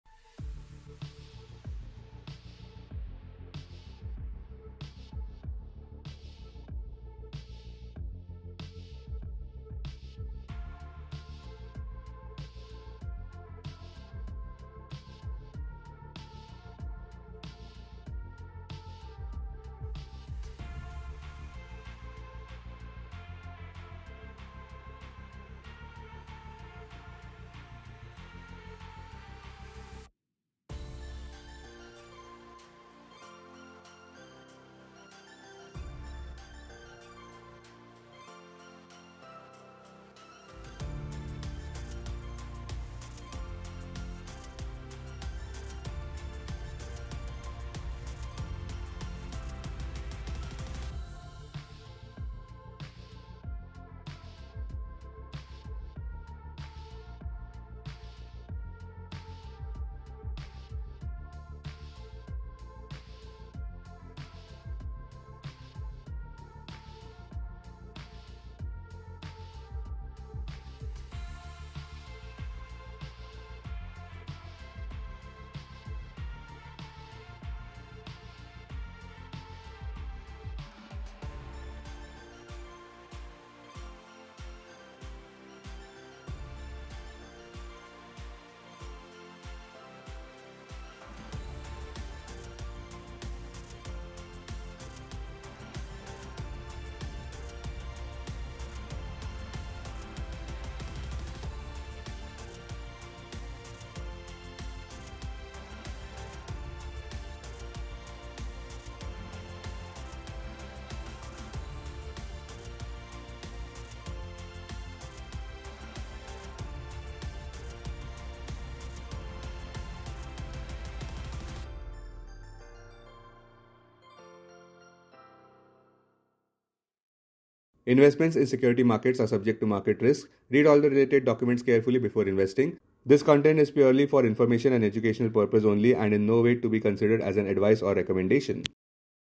📉Sensex down 168 pts, Nifty below 25,000; Realty, Media shines; FMCG drags
In this video, learn about the buzzing stocks, sectors that are growing, and much more.